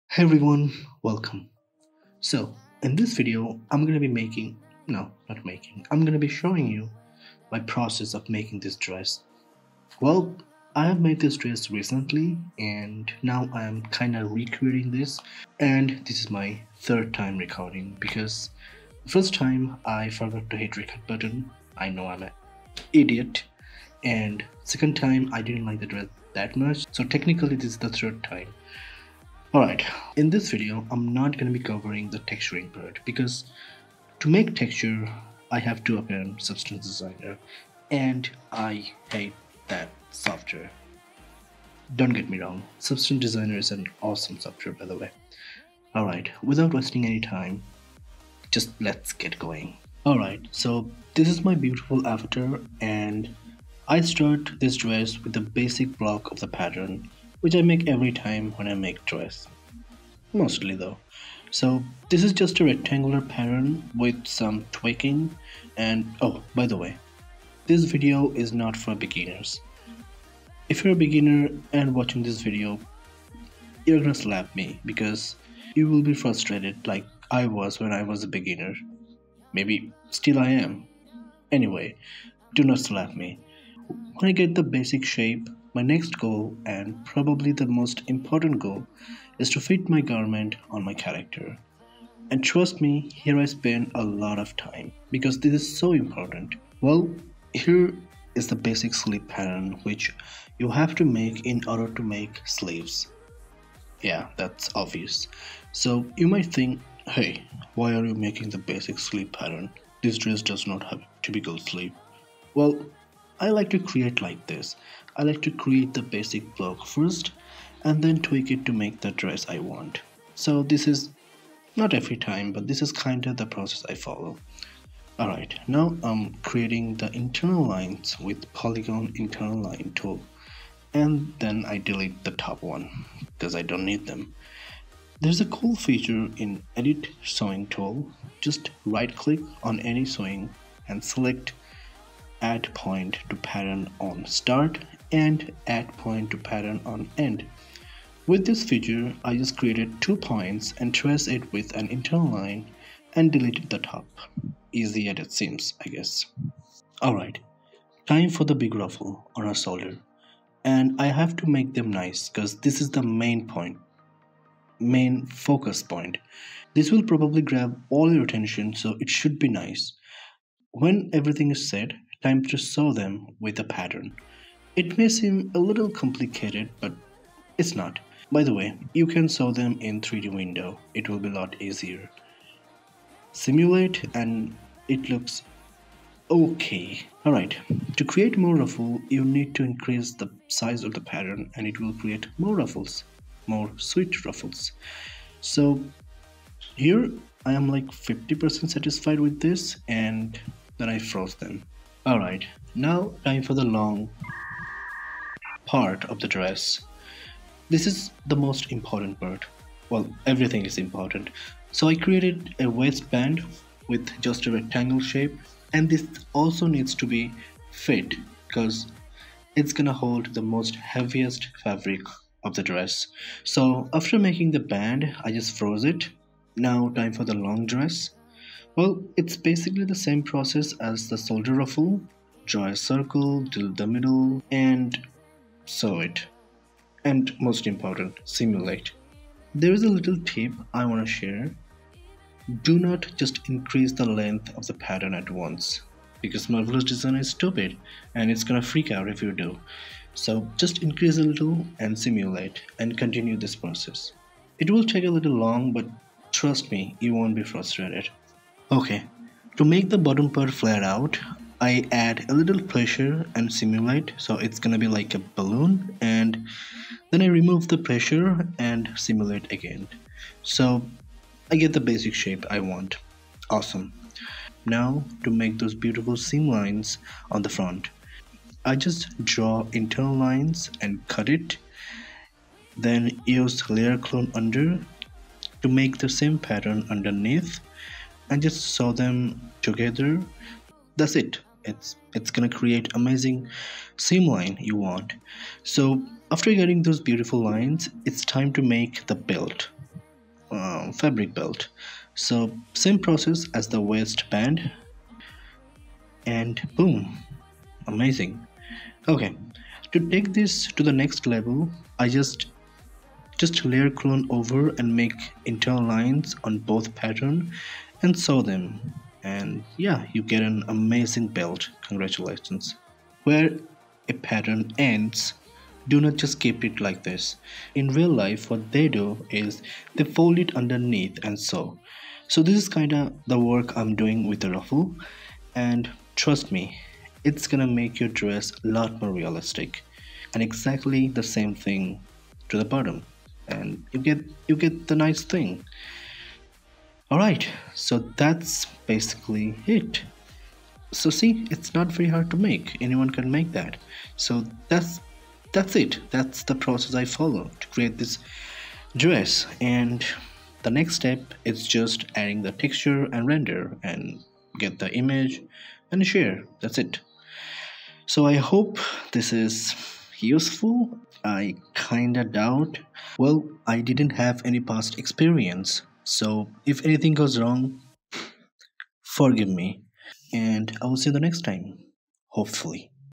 Making a Female Dress in Marvelous Designer/Clo3D
just a process of making the dress.
Software: Marvelous Designer